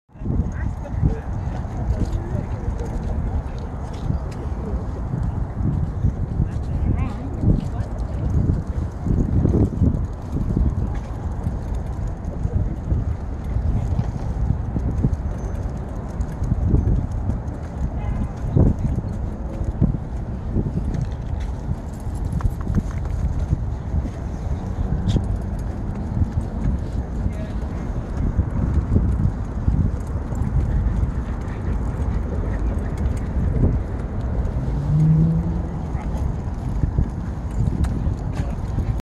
Cruising in my cart
We went out for a stroll by the waterfront in Georgetown. I got some sun and fresh air.